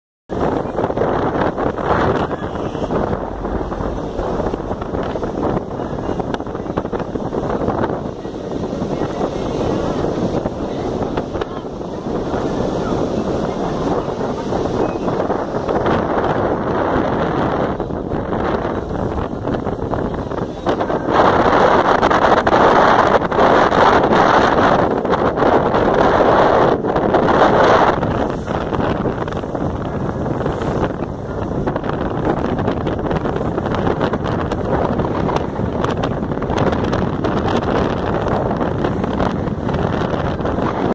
Darien, Lago Calima
podras encontrar deportes para practicar como un sano esparcimiento de lo hermoso que es Darien.